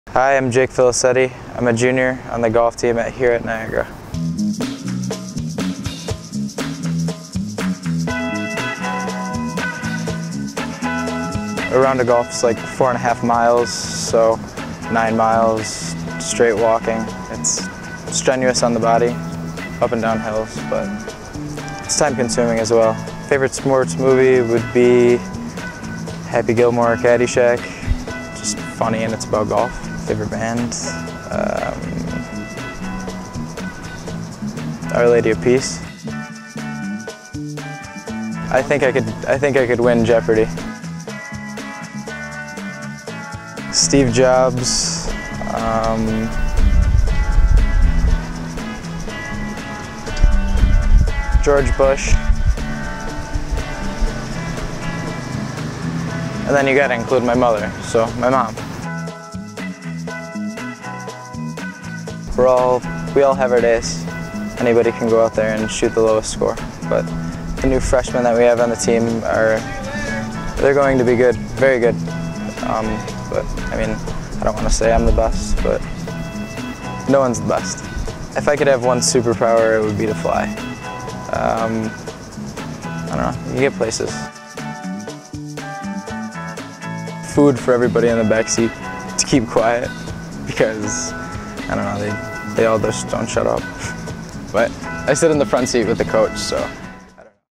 Eagle's Eye On: Jake Felicetti
Jake Felicetti from the men's golf team let's us in on his favorite sports movies (Spoiler: Surprise! They're golf movies) and who is the best golfer on the team.